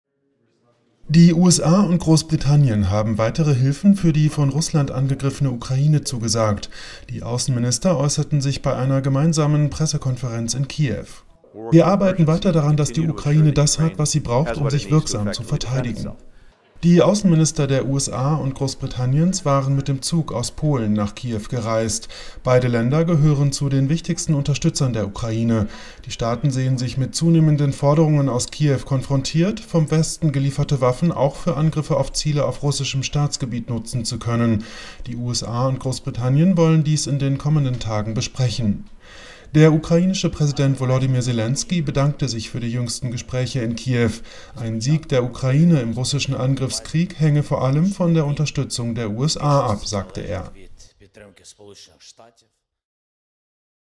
USA und UK sichern Ukraine weitere Waffen und Geld zu
Die USA und Großbritannien haben weitere Hilfen für die Ukraine zugesagt. "Wir arbeiten mit Dringlichkeit daran, weiterhin sicherzustellen, dass die Ukraine alles hat, was sie braucht, um sich wirksam zu verteidigen", sagte US-Außenminister Blinken in Kiew.